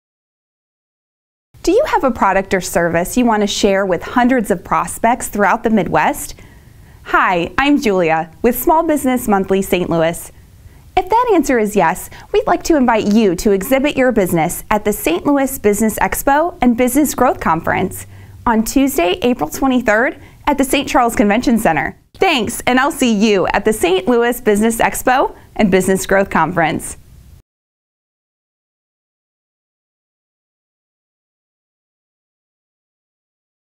See You at the St. Louis Business Expo & Business Growth Conference, April 23
The St. Louis Business Expo & Business Growth Conference is the No. 1 business conference and educational event in the St. Louis area. It is free to attend. Meet the area's top business leaders and learn how to take your company and career to another level. for more information and to register.